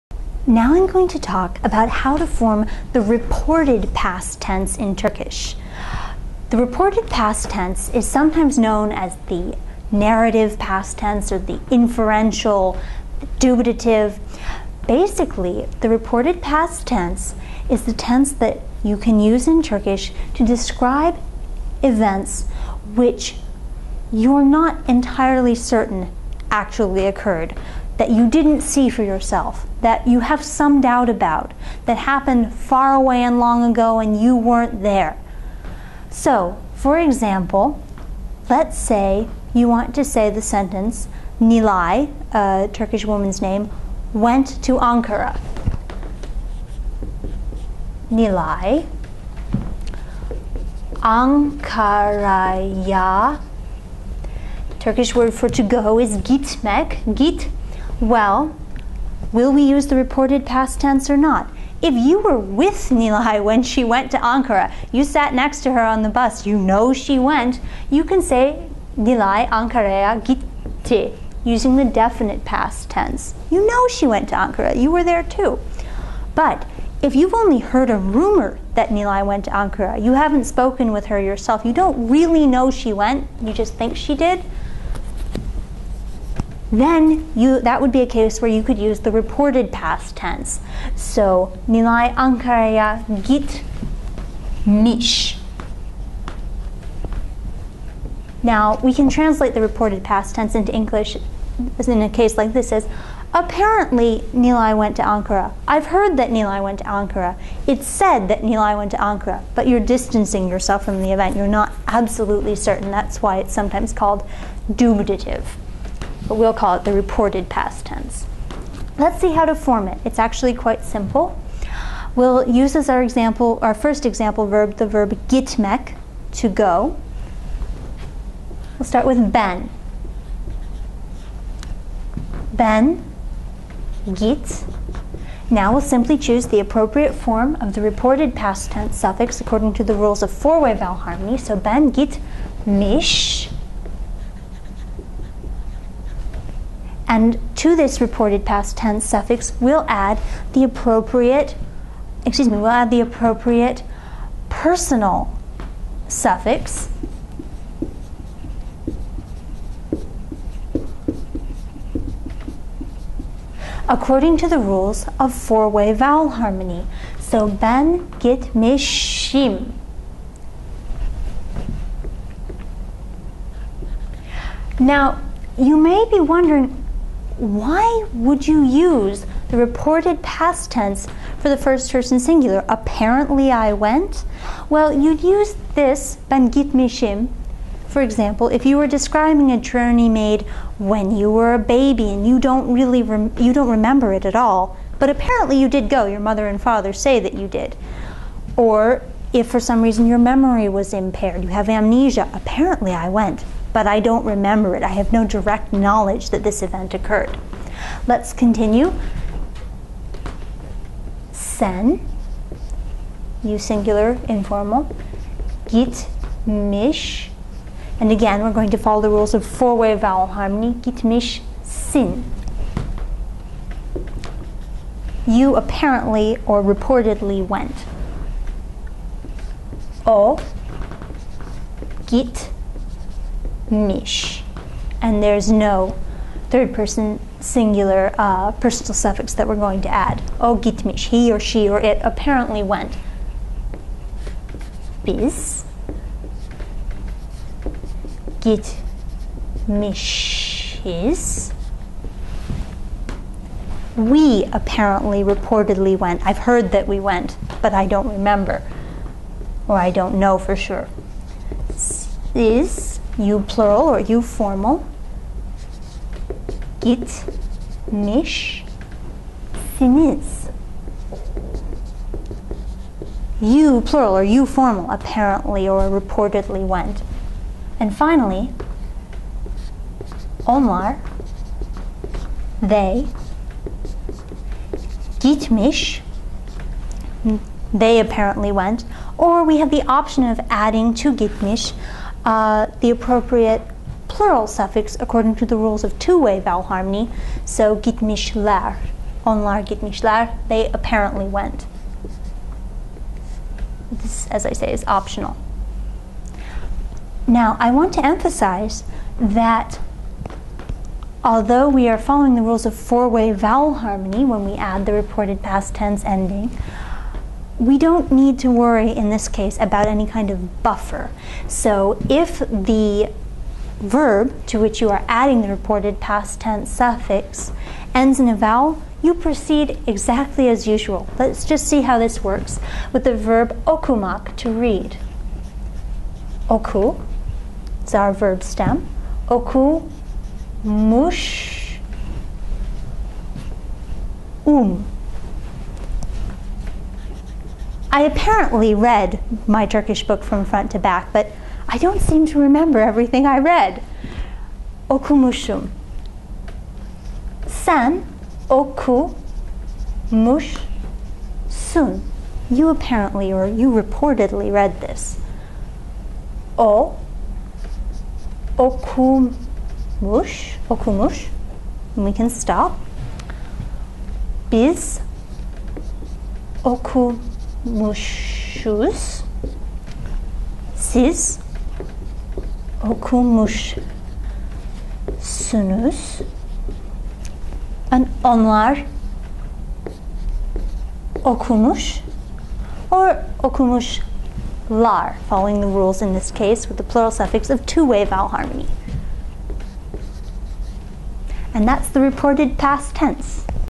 Turkish Grammar: Reported Past Tense (-miş) Affirmative Statements
How to say “He (apparently) went”, Reported Past Tense (-miş) Affirmative Statements